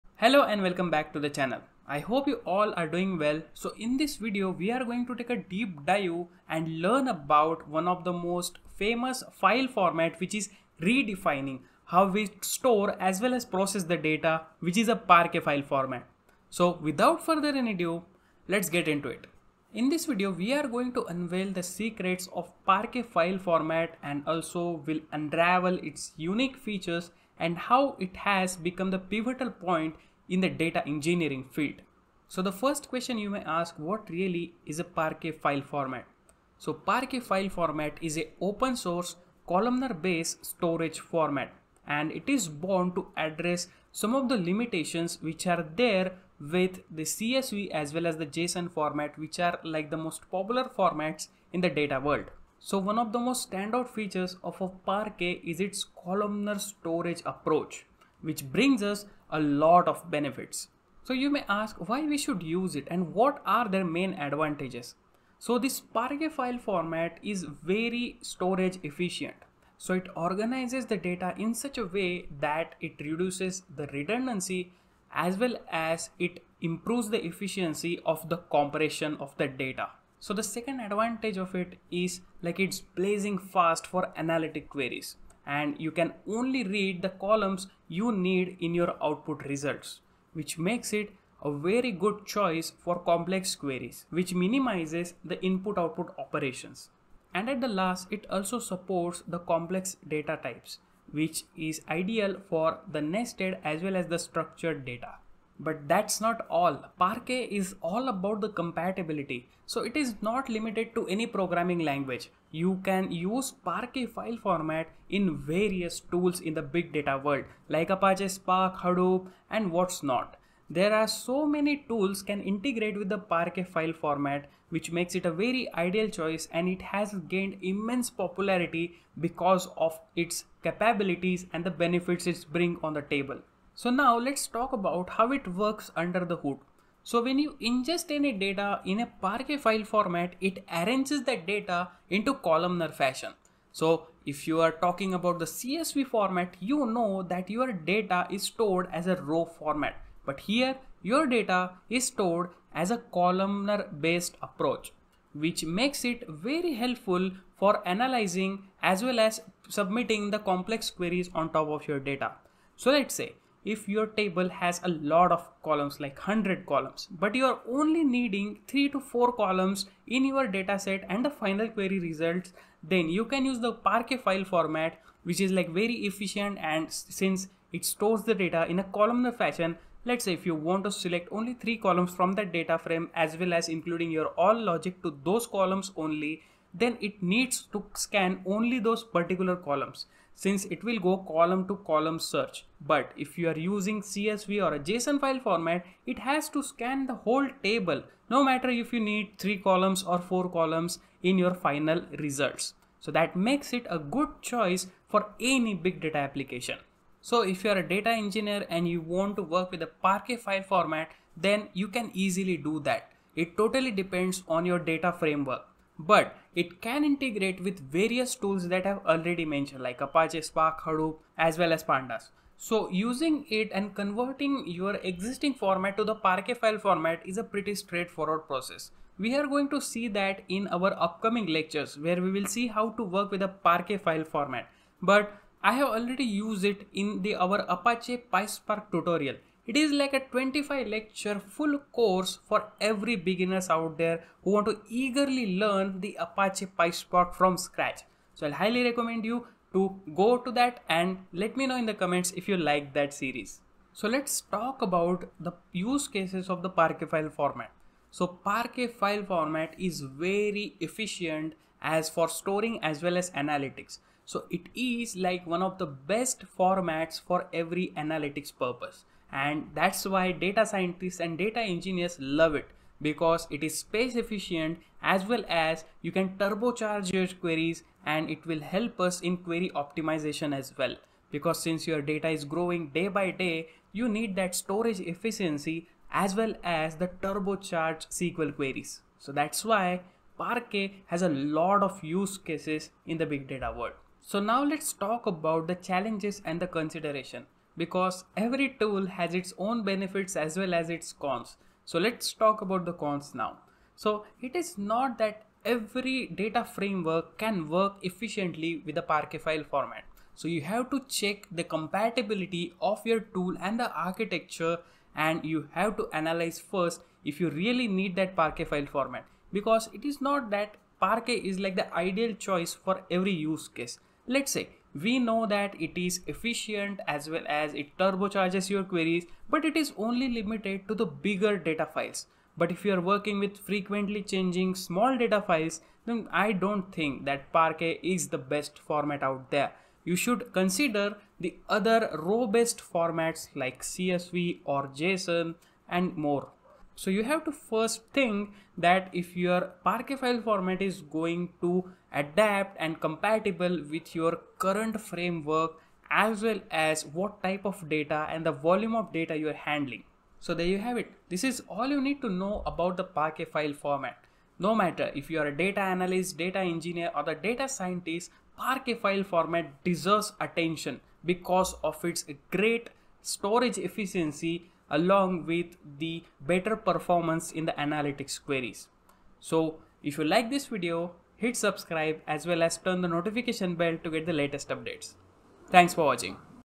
What is Parquet File Format?
In 'What is Parquet File Format?,' we delve into one of the most efficient and popular file formats for big data. 🚀

Discover the ins and outs of Parquet, an open-source columnar storage format that's revolutionizing the way we handle data. 🗂️🔍

Learn how Parquet optimizes data storage, improves query performance, and simplifies data processing, making it a favorite choice for data engineers and analysts. 💡

In this video, we'll demystify the Parquet file format, explore its benefits, and showcase real-world use cases. Whether you're new to data engineering or an experienced pro, understanding Parquet is a must!

Don't miss this opportunity to enhance your data knowledge and stay ahead in the data-driven world. Watch now to uncover the secrets of Parquet! 📈🔗

Check out our full course topic wise playlist on some of the most popular  technologies:

Channel Description-